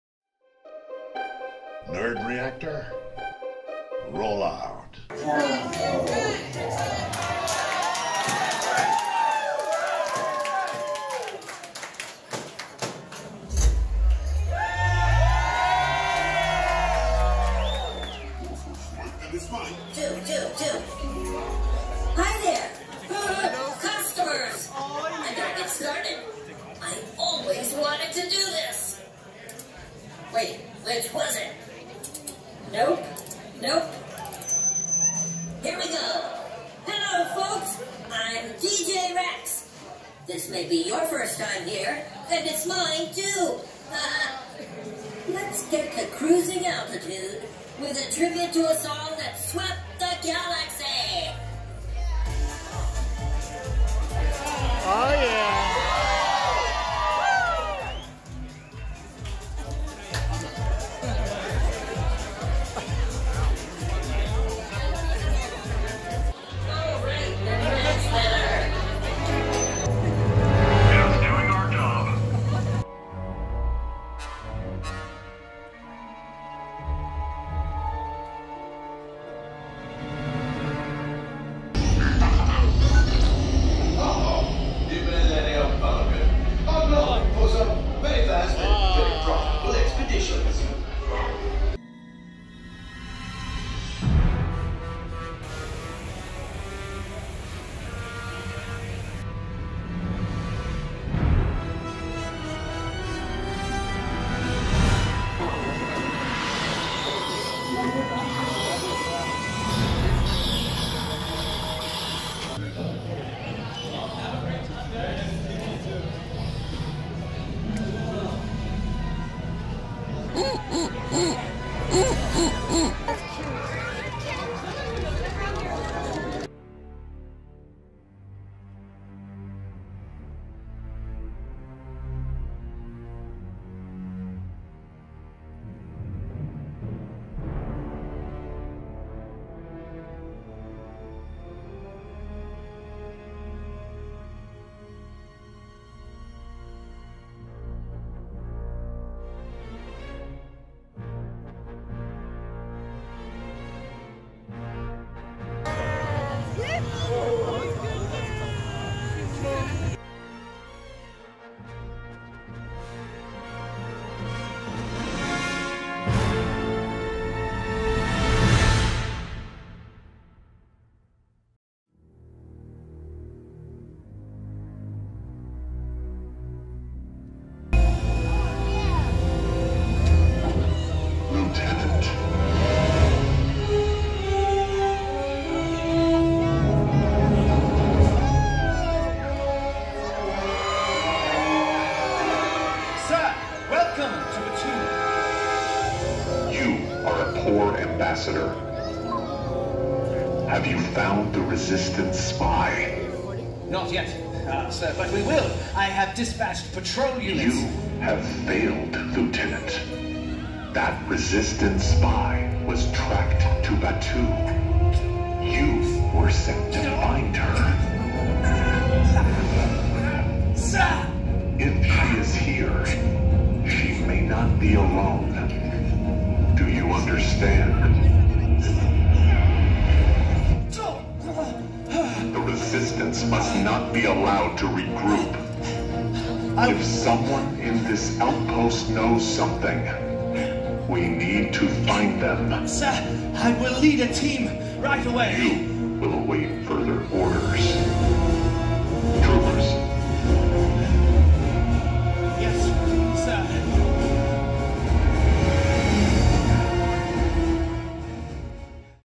Random Star Wars: Galaxy's Edge Adventures at Disneyland
Nerd Reactor checks out Galaxy's Edge at Disneyland.